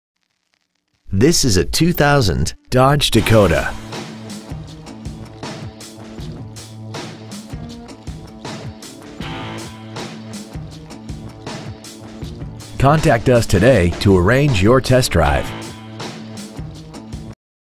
Used 2000 Dodge Dakota Paso Robles CA
We've been honored to serve the Paso Robles CA area , we promise that your experience at our dealership will exceed your expectations !

 Year : 2000
 Make : Dodge
 Model : Dakota
 Engine : 4.7L V8

 Exterior : Silver
 Miles : 83,881
 Interior : Gray
 Stock : 10078